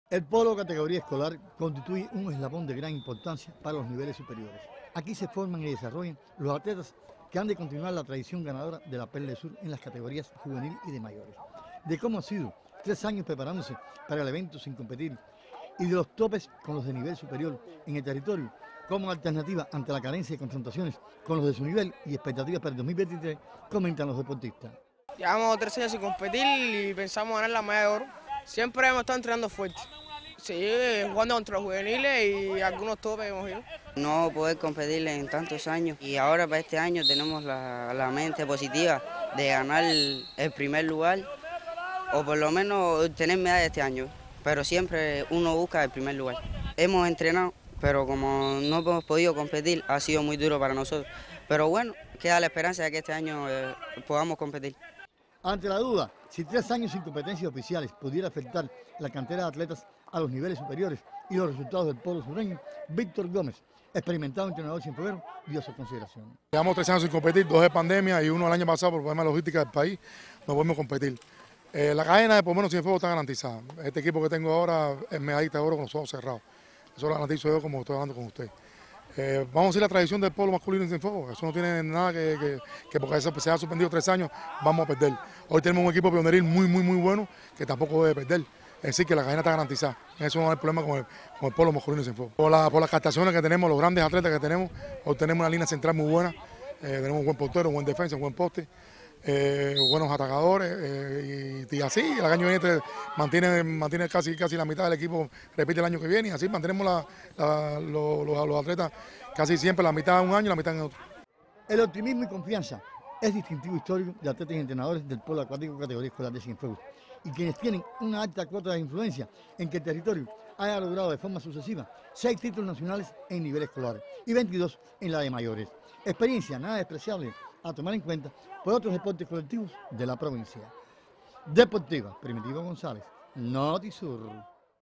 En preparación polistas de Cienfuegos con vista a Juegos Escolares Nacionales
Los polistas escolares de Cienfuegos no cesan en su preparación con vista a los Juegos Nacionales de la disciplina, por efectuarse en la llamada Perla del Sur.